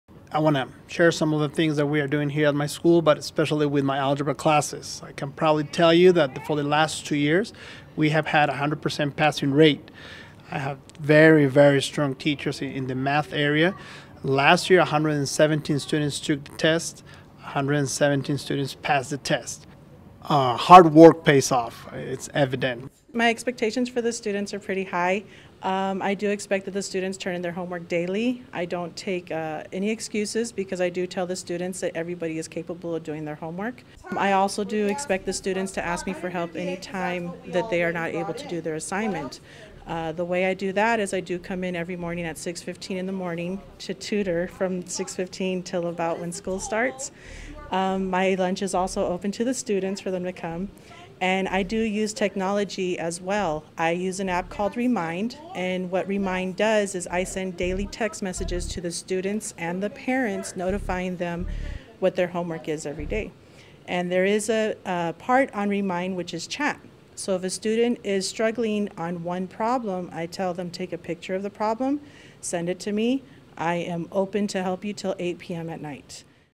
Morehead Math
Morehead math students have a 100 percent passing rate thanks to their hard work and their dedicated teachers. Go Raiders!